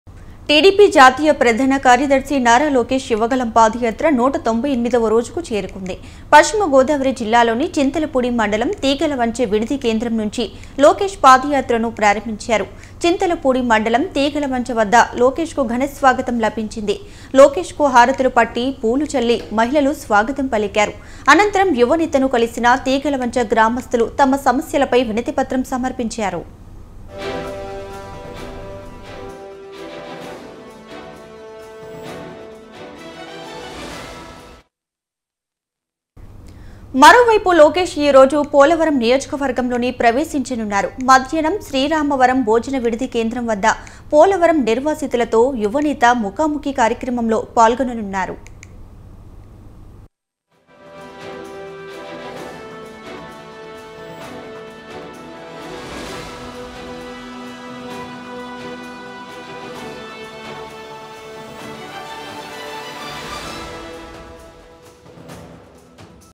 Nara Lokesh : తమ సమస్యలపై వినతి పత్రం ఇచ్చిన గ్రామస్తులు | TDP | Yuva Galam
Compensation to the Chintalapudi project displaced will be paid if the TDP is voted to power, TDP national general secretary Nara Lokesh promised the project displaced people.

The Chintalapudi lift irrigation project displaced from Pragadavaram met Lokesh at Velagapalli in Chintalapudi Assembly segment of the combined West Godavari district on Monday and complained that though they had sacrificed their lands for the lift irrigation scheme, the promised amount of Rs 30 lakh per acre has not been paid yet. As these lands are under the prohibited category they are neither able to mortgage their lands nor sell them, the farmers informed Lokesh.